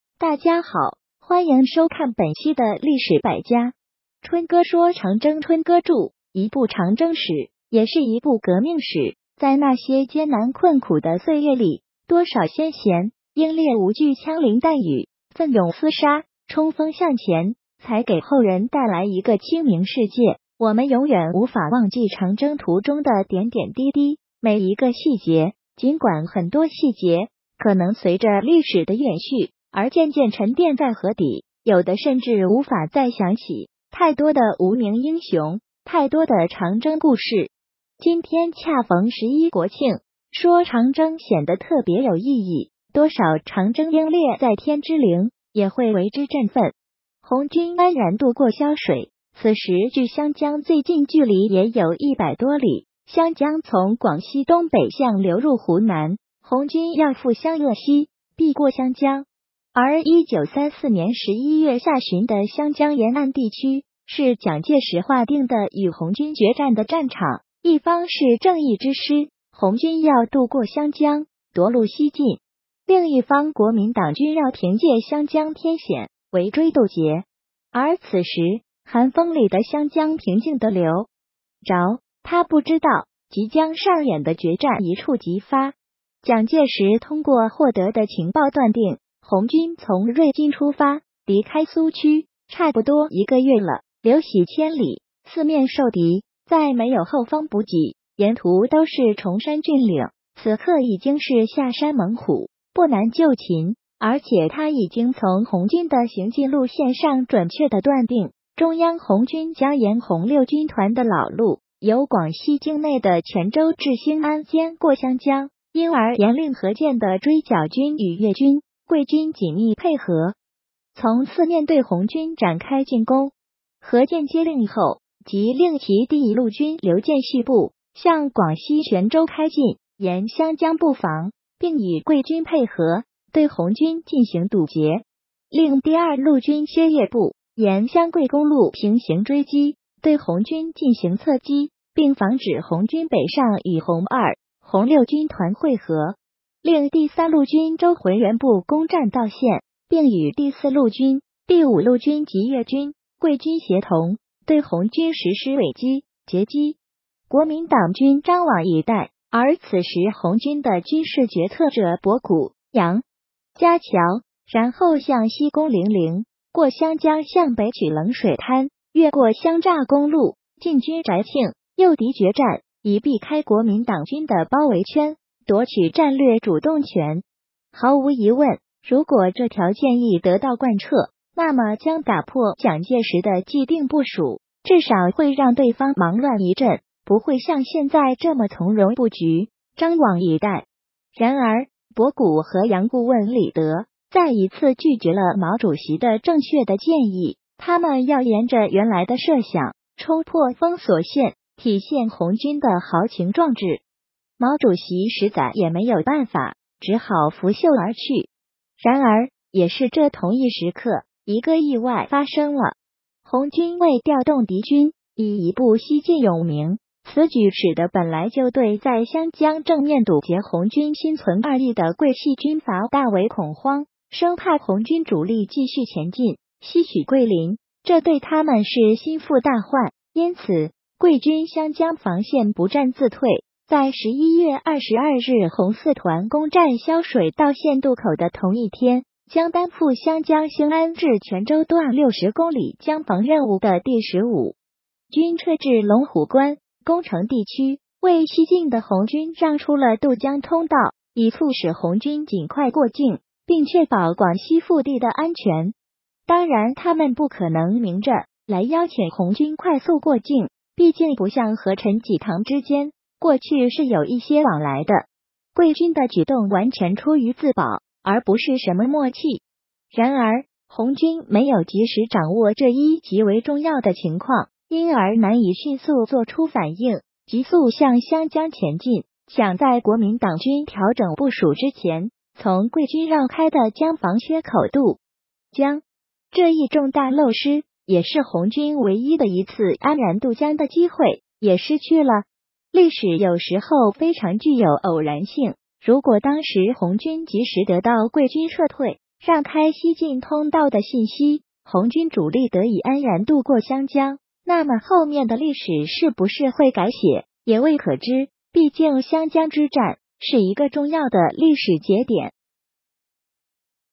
桂军曾经撤出120湘江防线，红军却错失渡江良机，究竟咋回事？
大家好,欢迎收看本期的历史百家.春哥说长征【春哥注：一部长征史，也是一部革命史，在那些艰难困苦的岁月里，多少先贤英烈无惧枪林弹雨，奋勇厮杀，冲锋向前，才给后人带来一个清明世界，我们永远无法忘记长征途中的点点滴滴，每一个细节，尽管很多细节可能随着历史的远去而渐渐沉淀在河底，有的甚至无法再想起，太多的无名英雄，太多的长征故事。今天恰逢十一国庆，说长征显得特别有意义，多少长征英烈在天之灵也会为之振奋。】红军安然渡过潇水，此时距湘江最近距离也有100多里，湘江从广西东北向流入湖南，红军要赴湘鄂西，必过湘江。而1934年11月下旬的湘江沿岸地区，是蒋介石划定的与红军决战的战场：一方是正义之师，红军要渡过湘江，夺路西进；另一方国民党军要凭借湘江天险，围追堵截。而此时，寒风里的湘江平静地流着，它不知道，即将上演的决战一触即发。蒋介石通过获得的情报断定，红军从瑞金出发，离开苏区，差不多一个月了，“流徙千里，四面受敌”，在没有后方补给，沿途都是崇山峻岭，此刻已经是“下山猛虎，不难就擒”，而且他已经从红军的行进路线上准确地断定，中央红军将沿红6军团的老路，由广西境内的全州至兴安间过湘江，因而严令何键的“追剿”军与粤军、桂军紧密配合，从四面对红军展开进攻。何键接令后，即令其第一路军刘建绪部向广西全州开进，沿湘江布防，并与桂军配合，对红军进行堵截；令第二路军薛岳部沿湘桂公路平行追击，对红军进行侧击，并防止红军北上与红2、红6军团会合；令第三路军周浑元部攻占道县，并与第4路军、第5路军及粤军、桂军协同，对红军实施尾击、截击。国民党军张网以待，而此时红军的军事决策者博古、洋顾问李德则要指挥红军与敌人拼个鱼死网破。身在第二路纵队随军步行的毛主席心急如焚，在前锋部队占领道县之后，他又提出建议，尽管上一次的建议没有被采纳，但毛主席并不是小鸡肚肠的人，他出于对革命事业的忠诚和责任，眼看面临重大危机的时刻，他必须再次说话。毛主席找到李德他们，给出的建议是：“红军应立即沿潇水西岸北上，攻占板桥铺、渔涛湾、华江铺、双牌、富家桥，然后向西攻零陵，过湘江向北取冷水滩，越过湘柞公路，进军宅庆，诱敌决战，以避开国民党军的包围圈，夺取战略主动权。”毫无疑问，如果这条建议得到贯彻，那么将打破蒋介石的既定部署，至少会让对方忙乱一阵，不会像现在这么从容布局，张网以待。然而，博古和洋顾问李德再一次拒绝了毛主席的正确的建议，他们要沿着原来的设想，冲破封锁线，体现红军的“豪情壮志”，毛主席实在也没有办法，只好拂袖而去。然而，也是这同一时刻，一个意外发生了。红军为调动敌军，以一部西进永明，此举使得本来就对在湘江正面堵截红军心存二意的桂系军阀大为恐慌，生怕红军主力继续前进，西取桂林，这对他们是心腹大患，因此桂军湘江防线不战自退，在11月22日红4团攻占潇水道县渡口的同一天，将担负湘江兴安至全州段六十公里江防任务的第15军撤至龙虎关、恭城地区，为西进的红军让出了渡江通道，以促使红军尽快过境，并确保广西腹地的安全。当然他们不可能明着来邀请红军快速过境，毕竟不像和陈济棠之间，过去是有一些往来的。桂军的举动完全出于自保，而不是什么默契。然而，红军没有及时掌握这一极为重要的情况，因而难以迅速作出反应，急速向湘江前进，抢在国民党军调整部署之前，从桂军让开的江防缺口渡江。这一重大漏失也是红军唯一的一次安然渡江的机会，也失去了。历史有时候非常具有偶然性，如果当时红军及时得到桂军撤退，让开西进通道的信息，红军主力得以安然渡过湘江，那么后面的历史是不是会改写，也未可知，毕竟湘江之战是一个重要的历史节点。